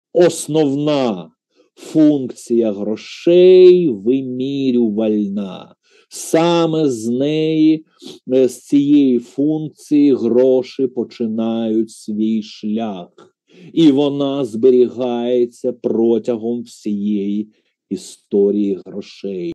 Про основну функцію грошей
Філософ Сергій Шевцов про основну функцію грошей.